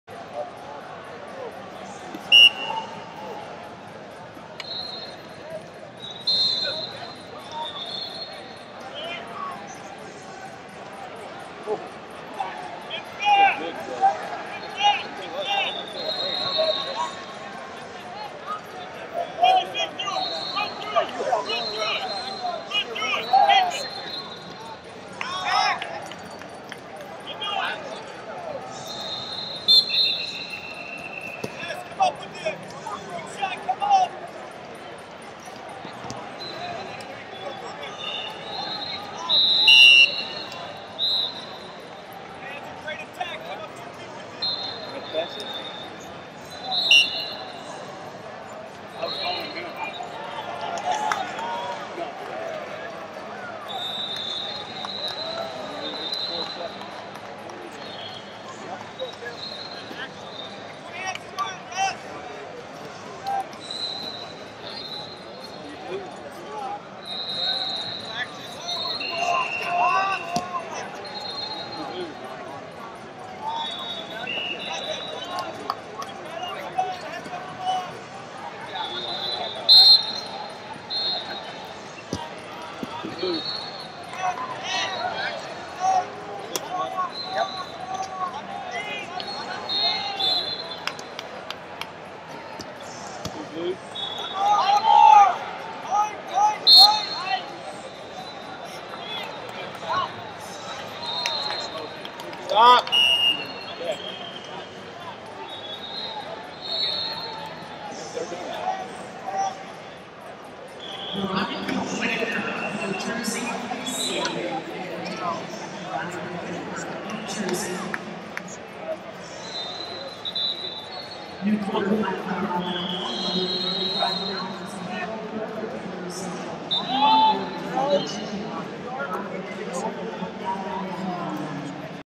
170 Lbs Cons 16 2 Chase Baczek Illinois Vs Gabriel Monroe New York
2019 USMC USAW Junior And Cadet National Championships - 170 Lbs Cons 16 2 Chase Baczek Illinois Vs Gabriel Monroe New York